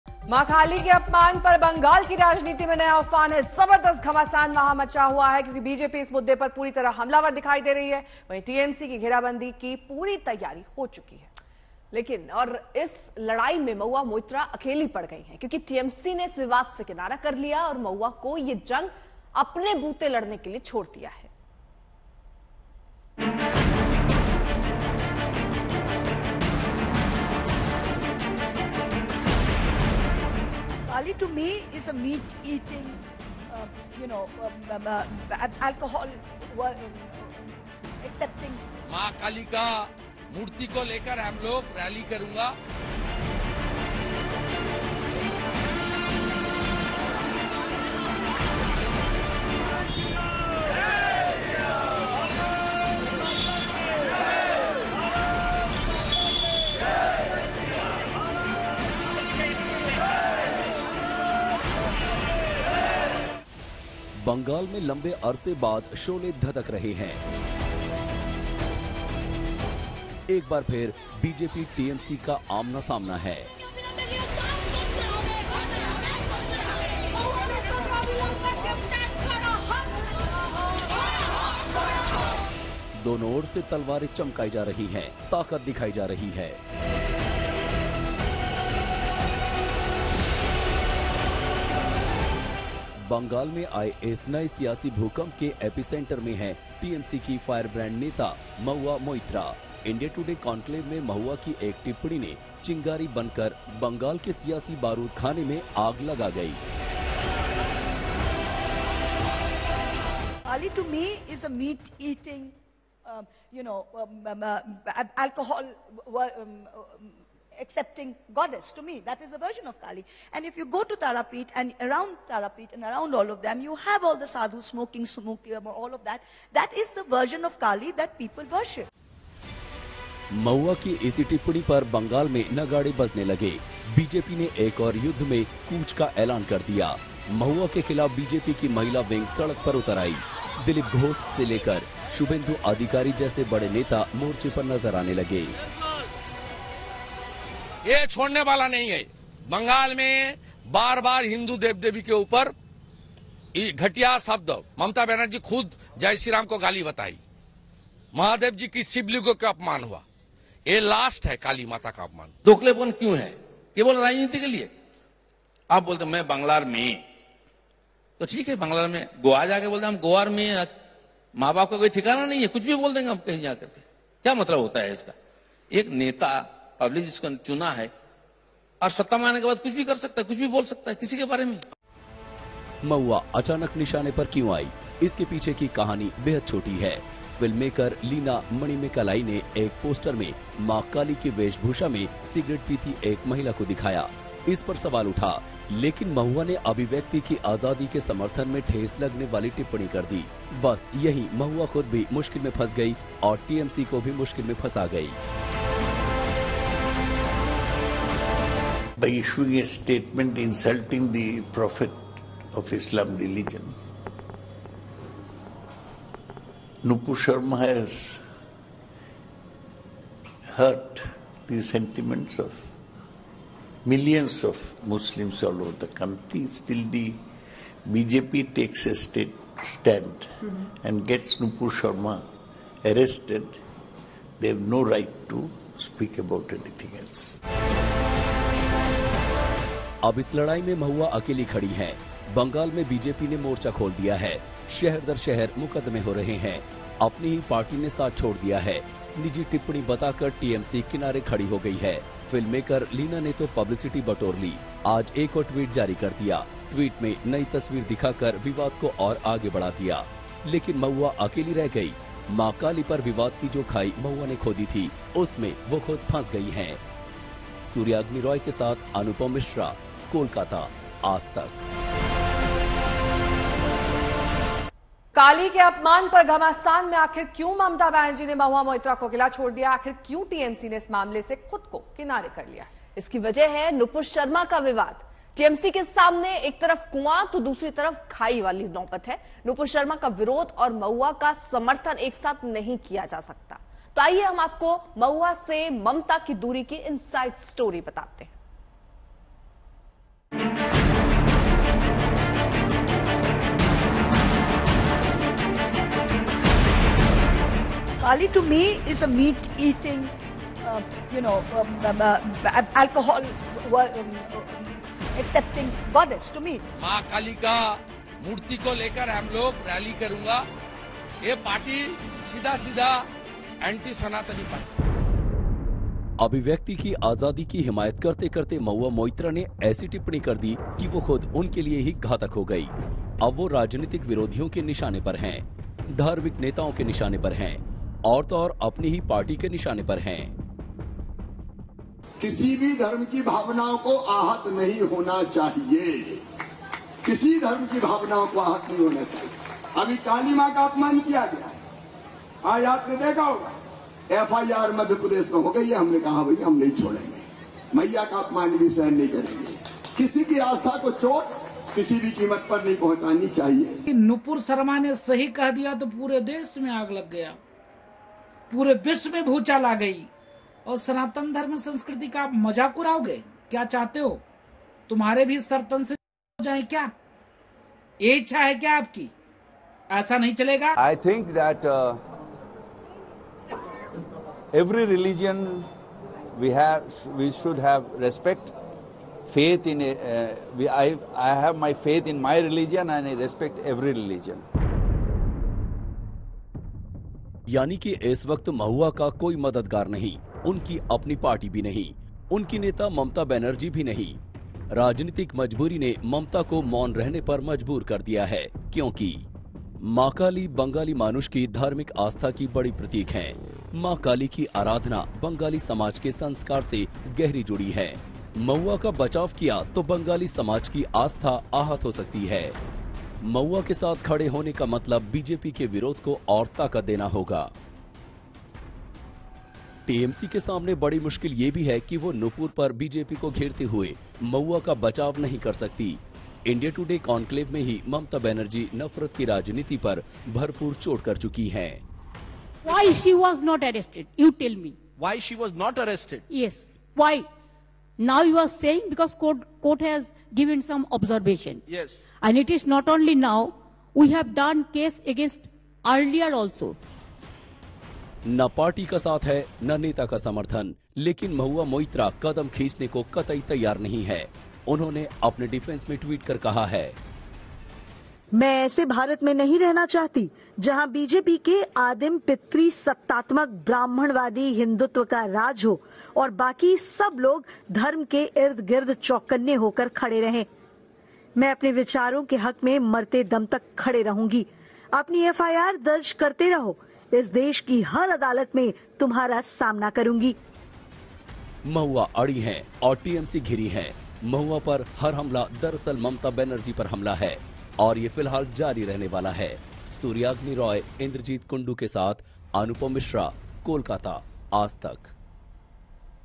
Kaali Poster Row: Mahua के बयान पर फंसी Mamata Banerjee, BJP ने ममता बनर्जी से पूछे कड़े सवाल
टीएमसी सांसद महुआ मोइत्रा मां काली को दिए बयान को लेकर फंसती नजर आ रही है. महुआ मोइत्रा के बयान पर बीजेपी ने अब ममता बनर्जी को घेरना शुरू कर दिया है. बीजेपी ने ममता बनर्जी से जवाब देने को कहा है.